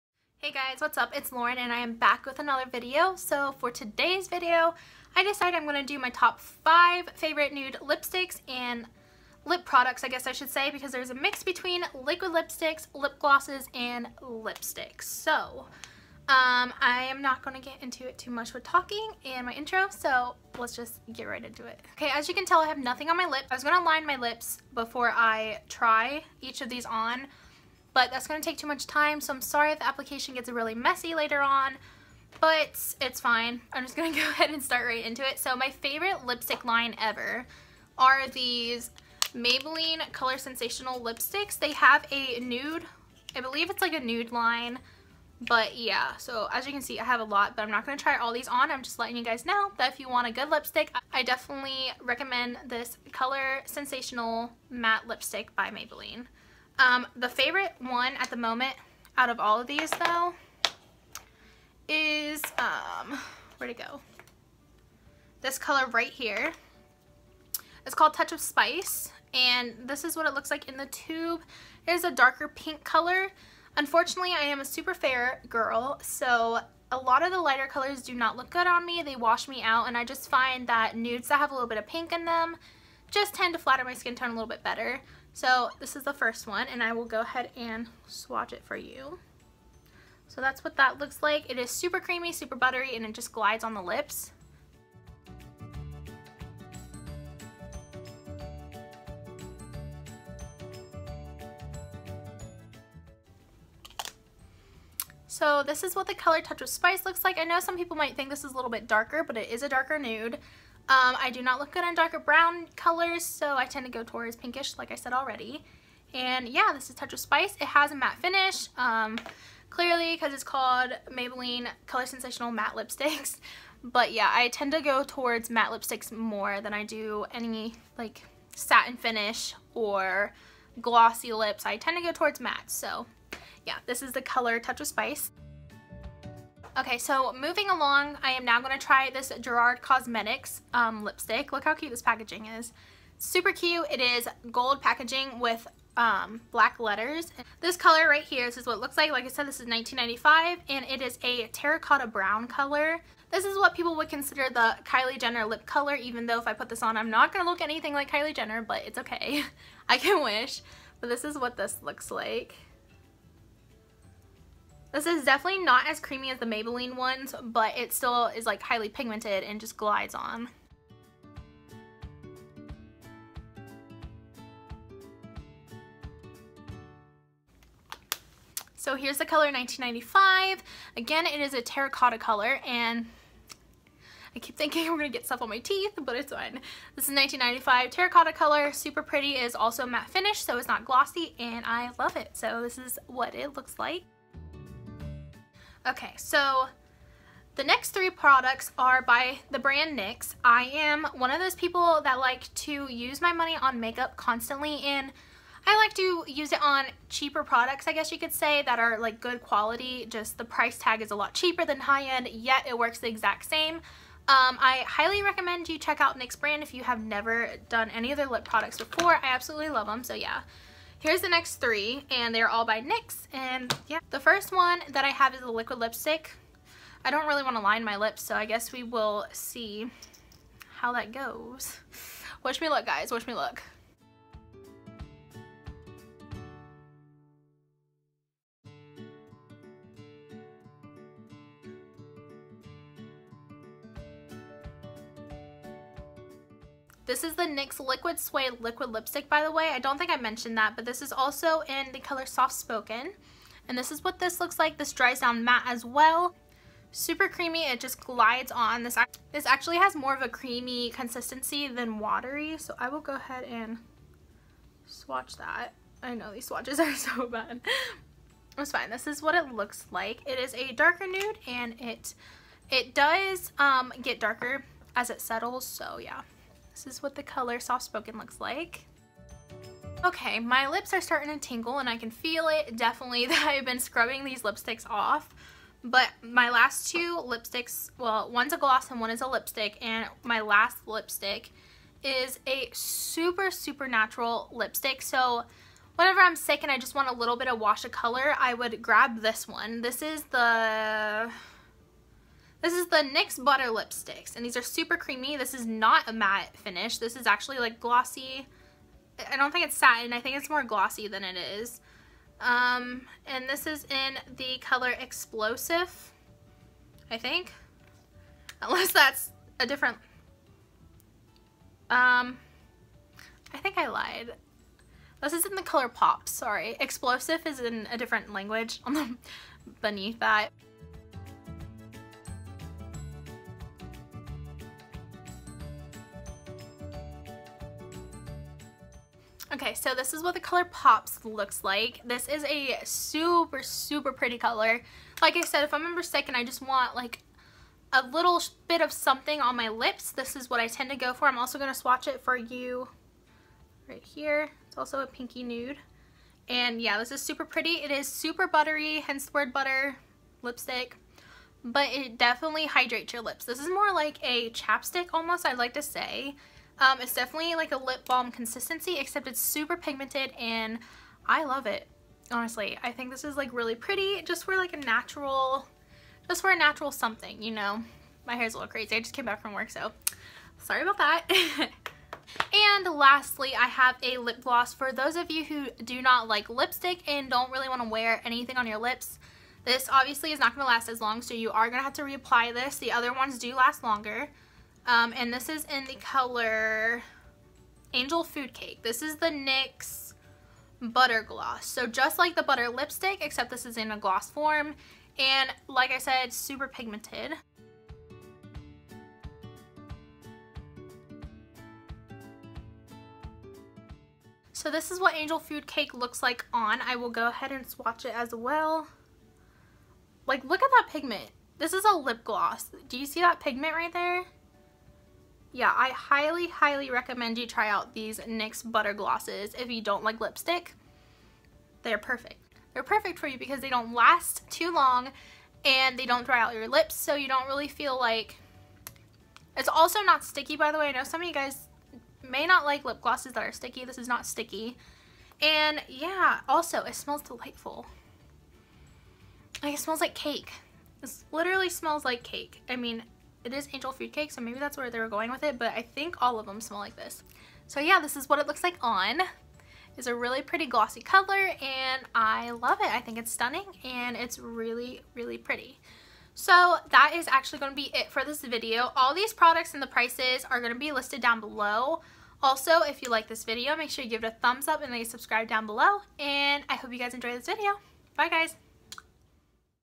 Top 5 Favorite Nude Lip Products!
Hey Guys! Since summer is coming I figured I'd share with you guys my favorite nude liquid lipsticks, lipsticks, and lip glosses! I hope you all enjoy!

xoxo,
Lauren

Products Mentioned:

*Maybelline Colorsensational Matte Lipstick in Touch of Spice- $5.50
*Gerard Cosmetics Lipstick in 1995- $19.00
*Nyx Butter Gloss in Angel Food Cake- $4.99
*Nyx Liquid Suede Liquid Lipstick in Soft-Spoken- $6.99
*Nyx Butter Lipstick in shade Pops- $6.00
(unfortunately I can't find this lipstick anywhere, but they do have a ton of other colors that I absolutely adore as well, sorry guys!!!)